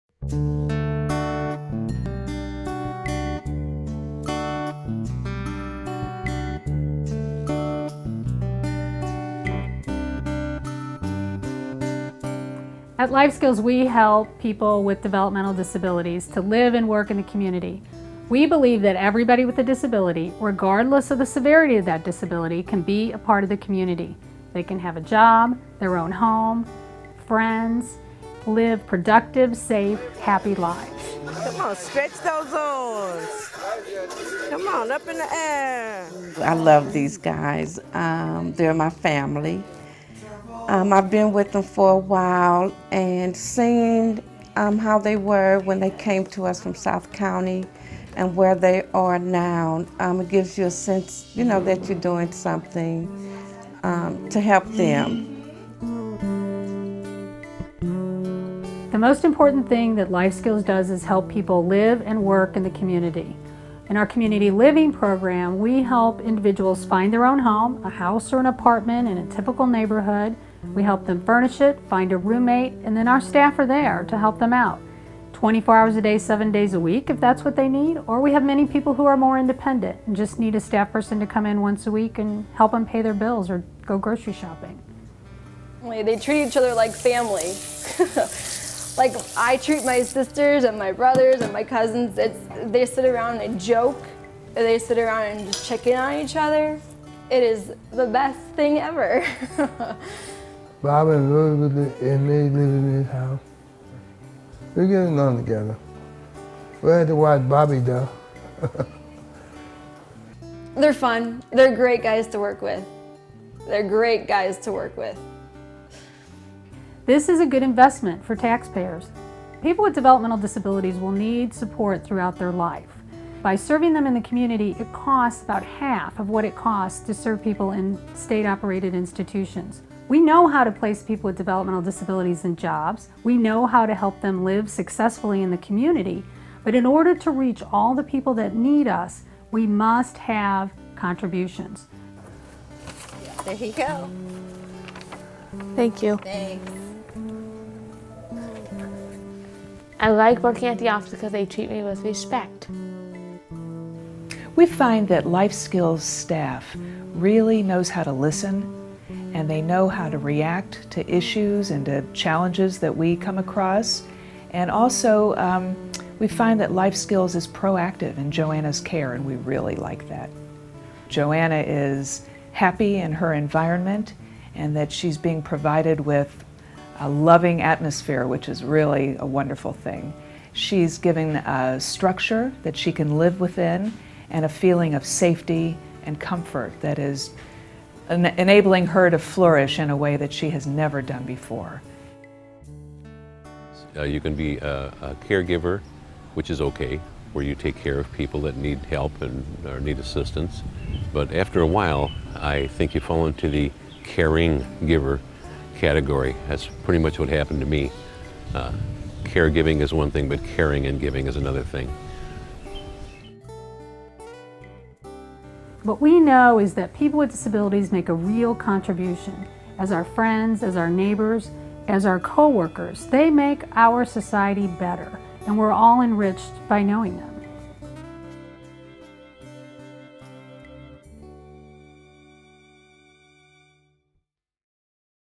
Life Skills-assisting people with disabilities live and work in the community
Life Skills is a St. Louis, Mo. based foundation that assists people with developmental disabilities to live and work with dignity in our community.
For more than 44 years Life Skills has connected individuals with disabilities to the St. Louis Community. Today, Life Skills serves more than 1,400 adults and teens each year to live in their own home and apartments, to find and keep a job, and to make friends and access the St. Louis community and all it has to offer.
If you would like more information on making a donation or how you can support your specific interests, you may contact Bill Eastman, Vice President of Development and Communications, at or call , ext. 1264.